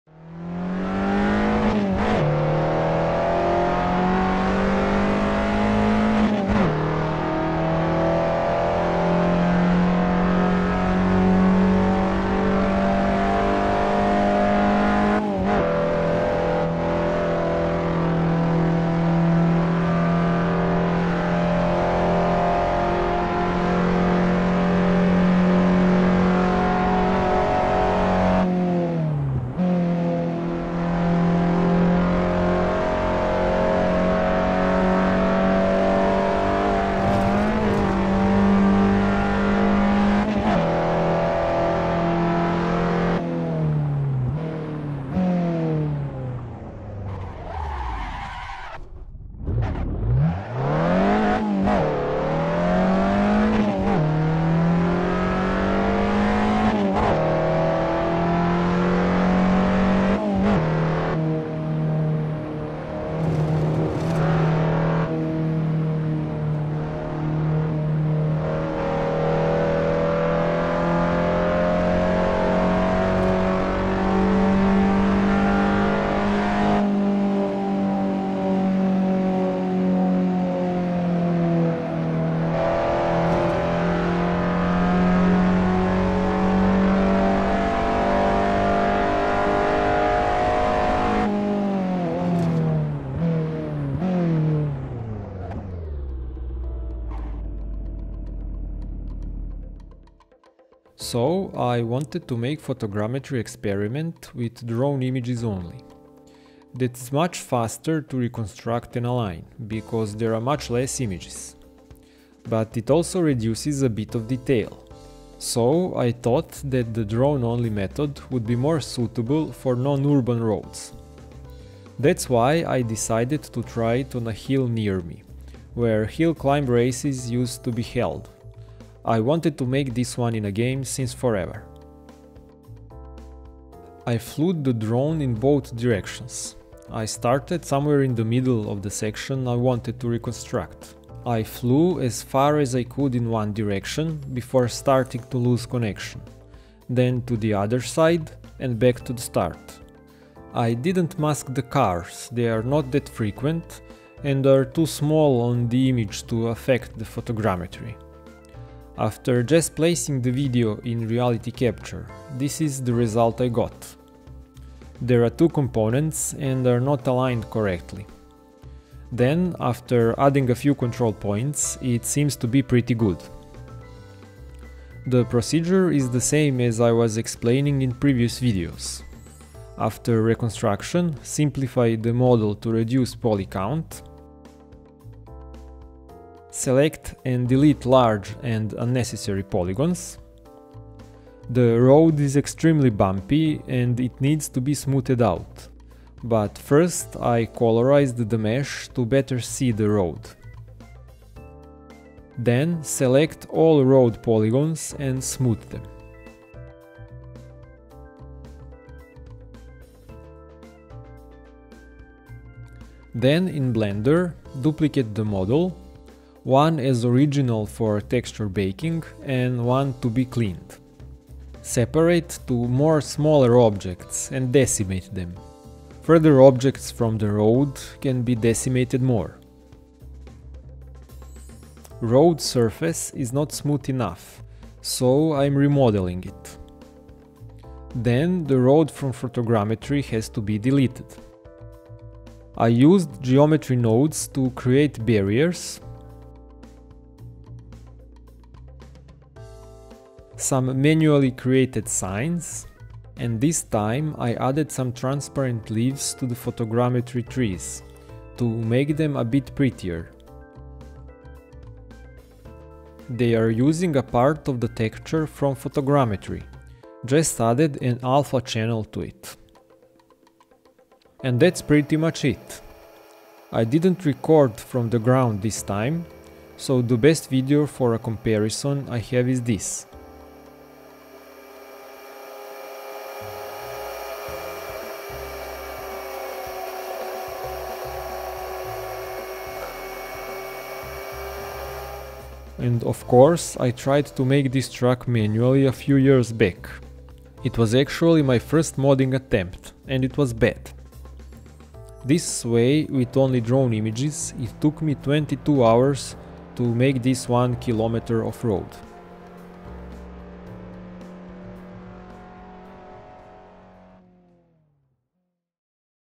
Hillclimb photogrammetry track, test with drone only
I wanted to make photogrammetry experiment with drone only images. That is much faster to reconstruct and align, because there are much less images. I made a Tresibaba hill near me, where hill-climb races used to be held.

I am not providing a download link because this experiment track is too short and not that fun. If I manage to create the whole track, about 11 km, which I'm planing to do, I'll share that for sure.